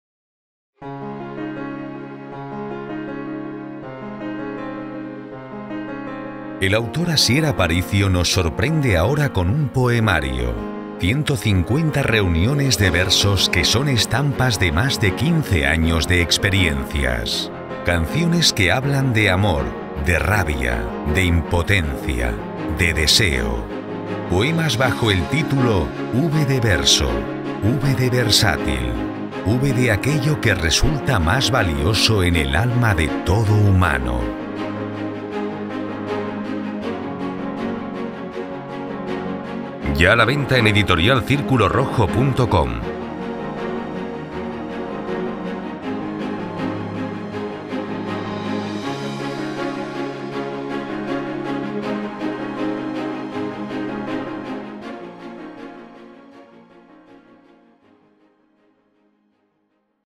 "V” de verso (Booktrailer) - Editorial Círculo Rojo
“V” de verso… También de “vida”, de “valor”, de “voluntad”… De
“vísceras”, de “violencia”, de “venganza”... “V” de “verdad” y “V”
de “veleta”... “V” de años de poesía, tan variados y contradictorios
como la búsqueda que continúa, que proseguirá para quien no desee
traicionar su condición.
He aquí estas 150 “reuniones de versos” que no llegan a poemas,
sino a canciones o “ripios cantados”. Estampas de momentos vividos
que a nadie importan… salvo por tener “algo de humanos”.